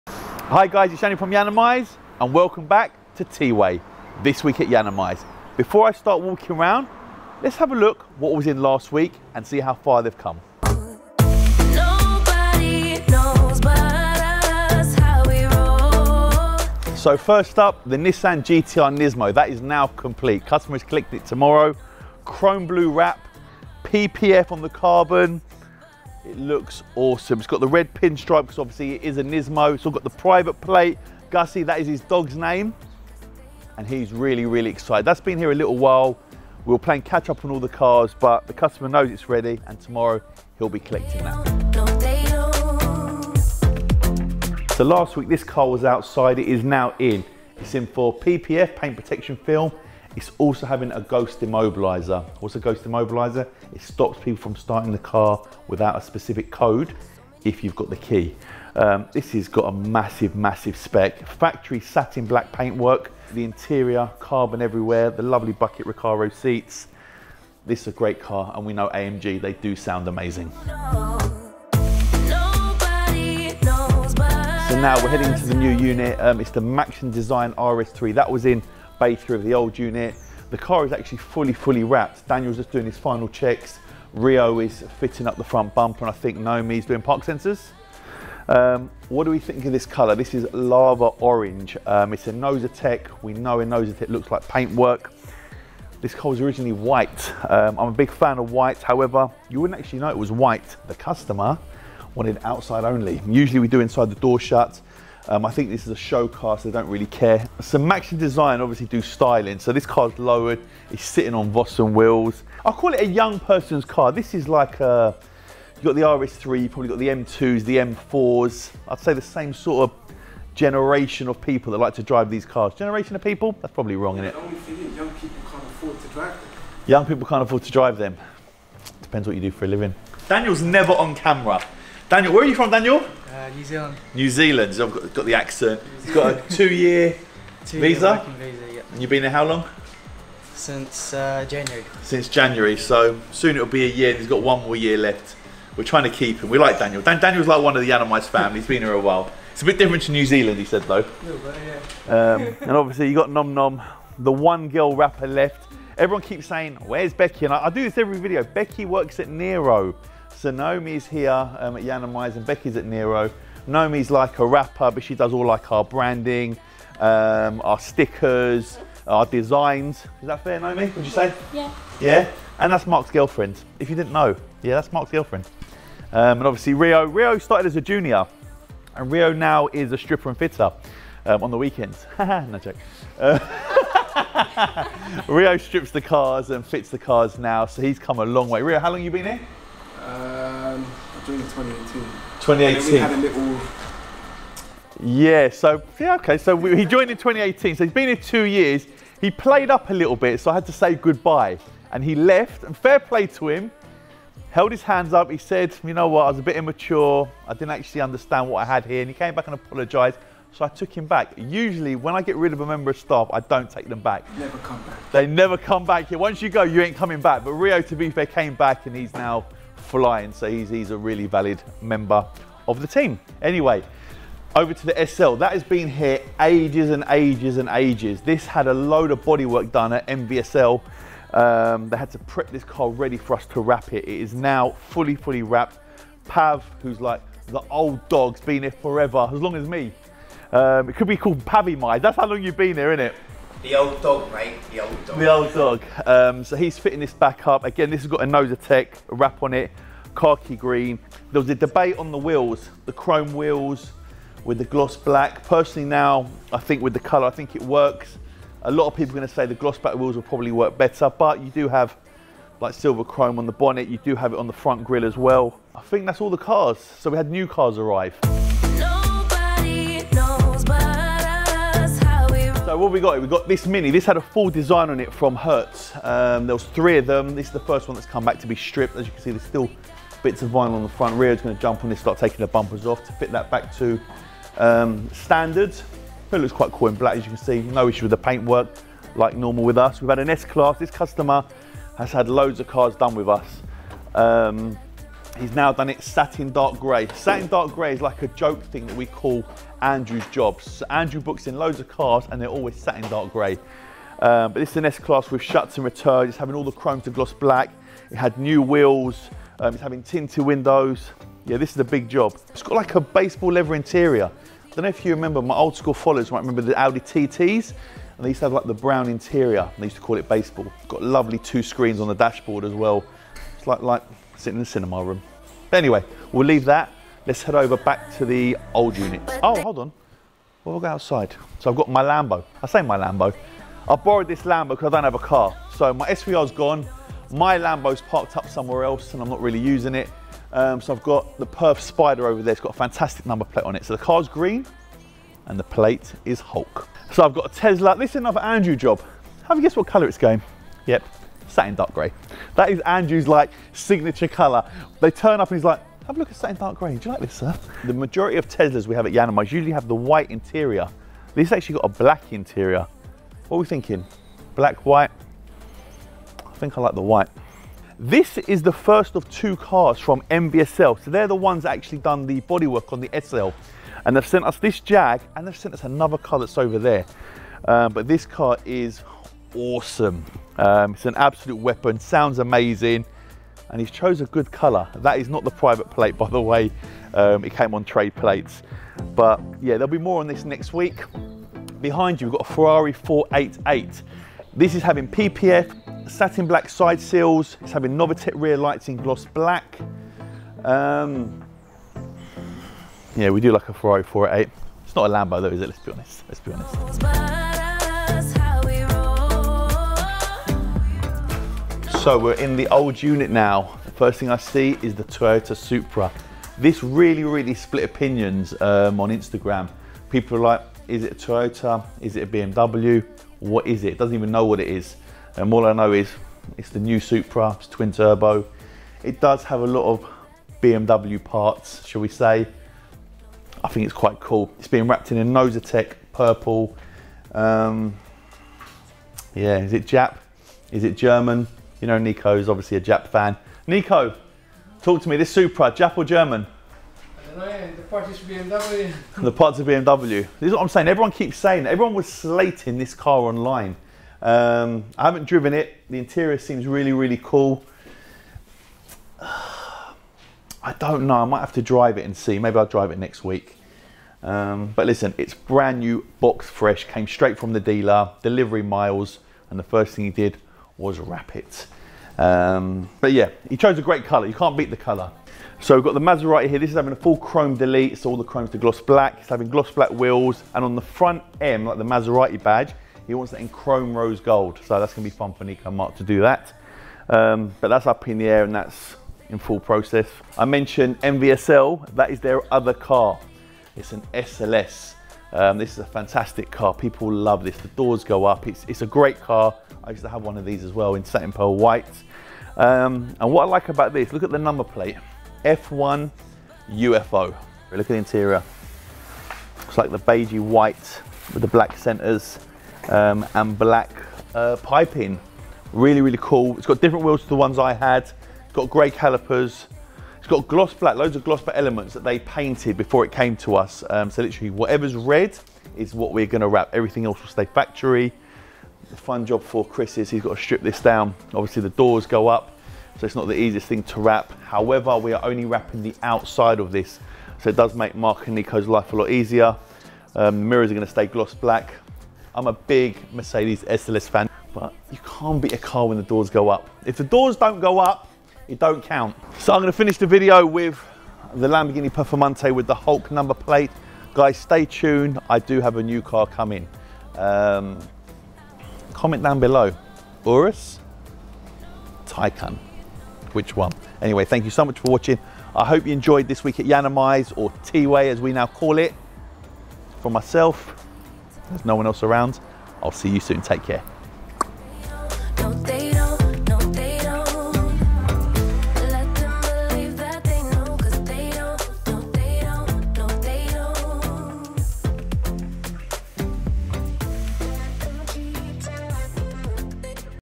Is This My New Incredible Car? TWAY ep11
So this week at Yiannimize we finally get to talk a little to Wrapper Daniel, a Toyota Supra wrapped Inozetek Purple, new Mercedes SLS being wrapped Chrome Red, a Lava Orange RS3 and I kinda sort of reveal my new car. :)

(00:00) Intro
(00:17) Nissan GTR Nismo - Chrome Blue Wrap
(00:48) Mercedes C63 AMG - PPF
(01:18) Audi RS3 - Inozetek Lava Orange
(03:45) Mercedes SL - Inozetek Gloss Khaki Green
(04:33) Mini Cooper » Full strip
(04:50) Mercedes S63 - Satin Dark Grey
(05:53) Tesla Model 3 - Satin Dark Grey
(06:09) Jaguar XK-RS - Satin Pearl White
(06:33) Ferrari 488 - Full PPF
(06:57) Toyota Supra - Inozetek Purple
(07:57) Maserati - Chrome delete
(08:14) Mercedes SLS - Chrome Red